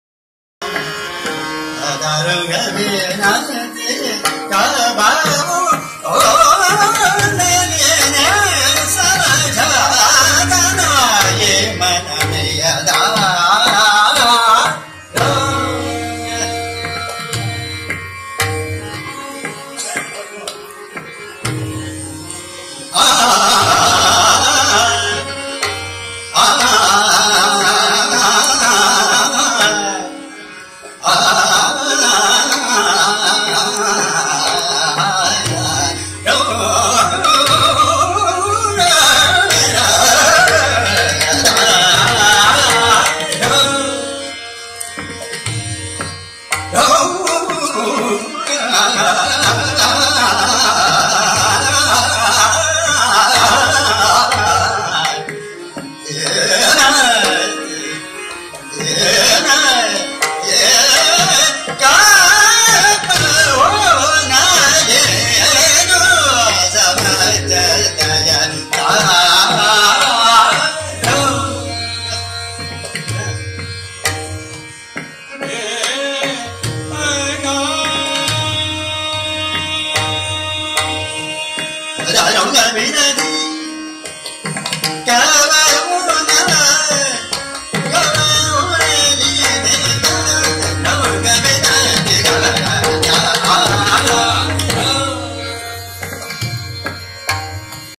Pandit Ulhas Kashalkar ji at Sawai SGBMF 2016
Pandit Ulhas Kashalkar ji at Sawai SGBMF 2016, Raga Nayaki Kanharaनायकी कान्हरा